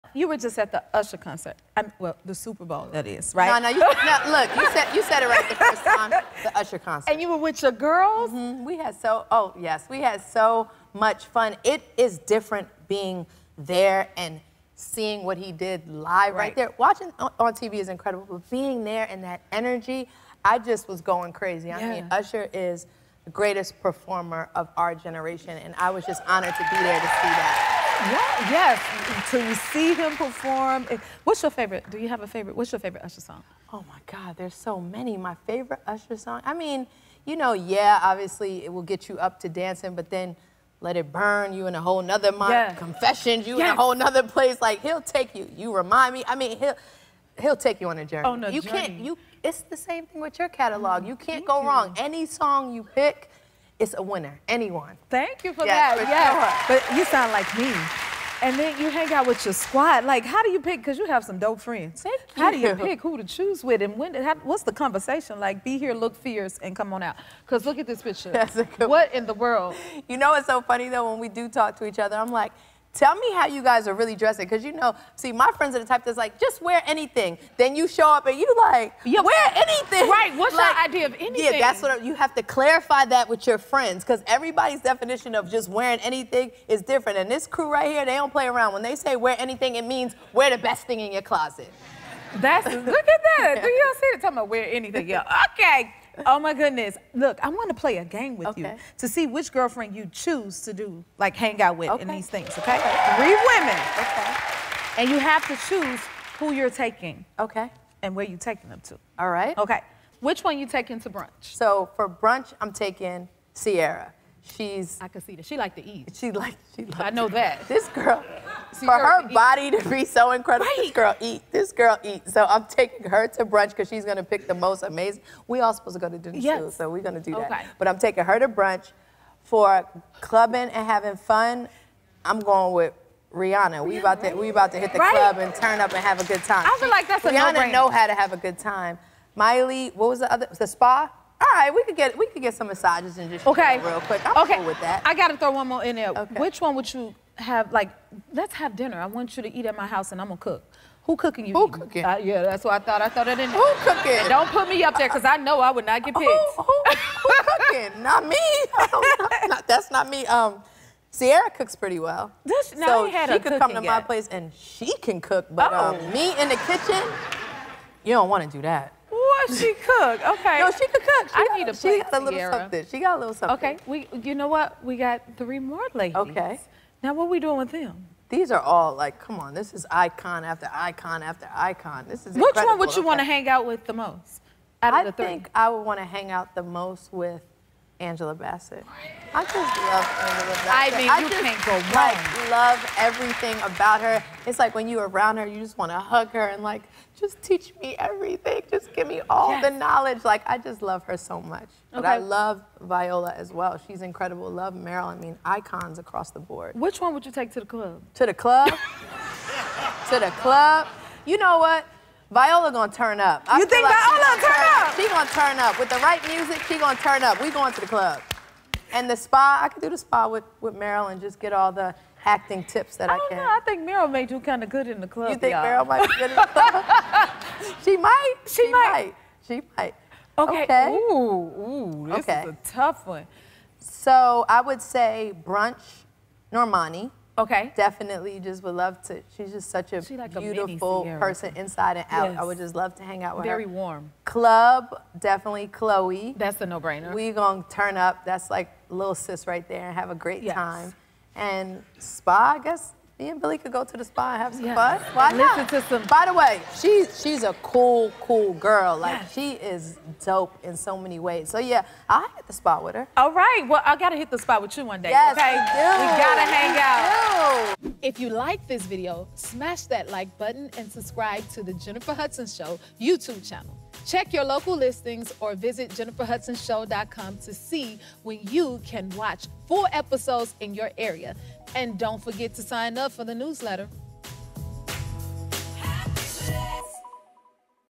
La La Anthony on Being in a Celebrity Friend Circle with Kim Kardashian & Ciara
La La Anthony reflects on attending the Usher concert. Then La La has a girlfriend moment with Jennifer and tells the host which celebrity she’d take to brunch, the spa, or club!